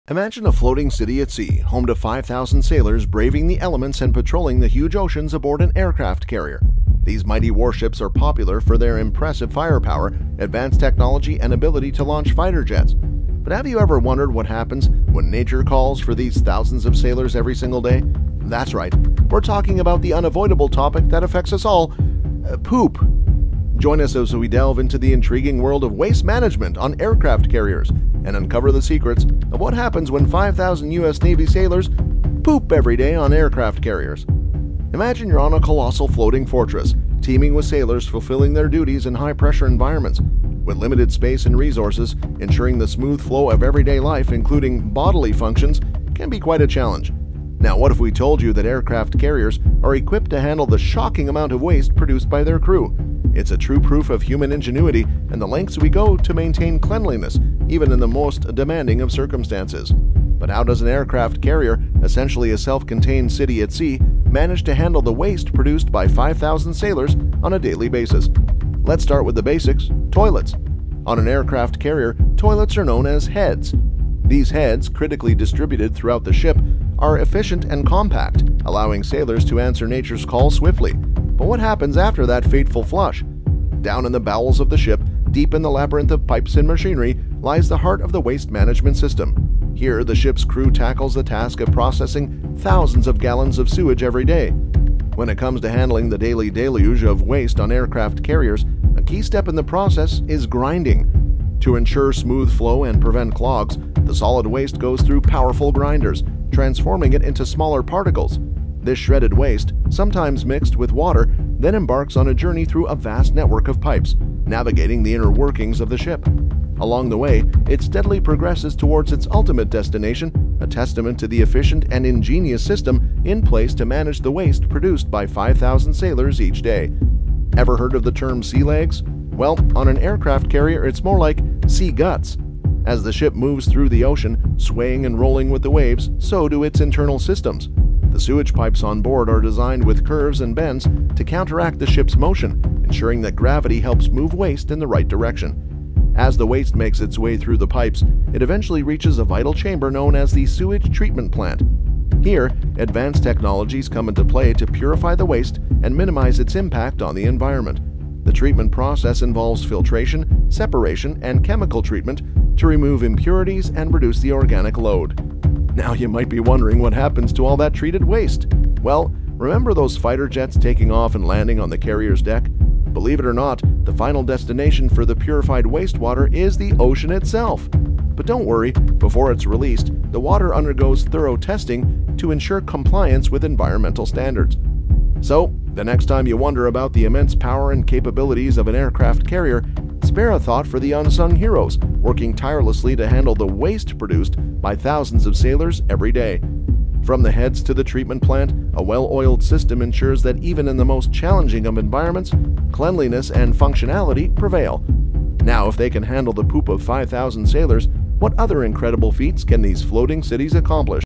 5,000 Sailors, One Aircraft Carrier: The POOP Management Mystery
5,000 Sailors, One Aircraft Carrier: The POOP Management Mystery

Imagine a vast city floating on the ocean, housing 5,000 sailors as they navigate the vast seas on an aircraft carrier. These formidable vessels are renowned for their formidable armaments, cutting-edge technology, and capacity to deploy combat aircraft. But have you ever considered the daily necessities of these thousands of sailors, particularly the call of nature? Indeed, we're addressing a universal concern: human waste. Join us as we explore the fascinating realm of waste management aboard aircraft carriers, revealing the intricacies of “What Happens When 5,000 US Navy Sailors POOP Every Day On Aircraft Carriers,” a blend of engineering marvel and essential sanitation solutions.